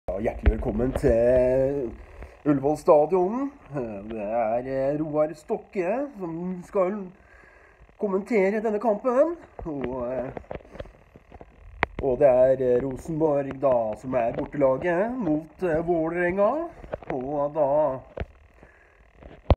Parodi av Roar Stokke
Parodi av fotball-kommentatoren Roar Stokke